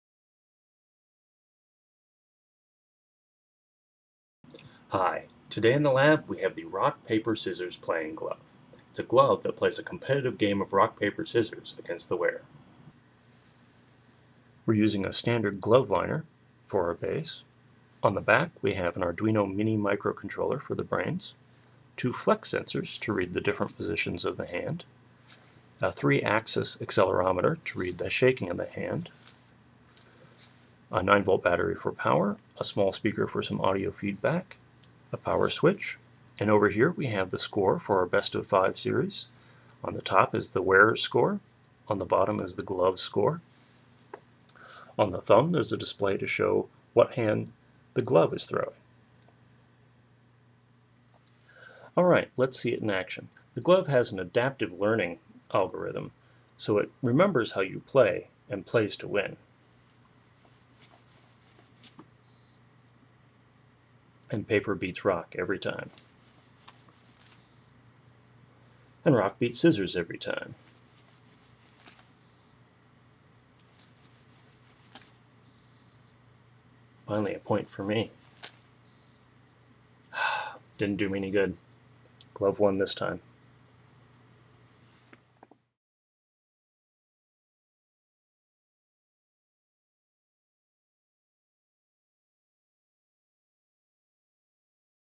Rock Paper Scissors Playing Glove
This glove plays Rock Paper Scissors against whoever is wearing it.  It learns how the wearer plays and it plays to win.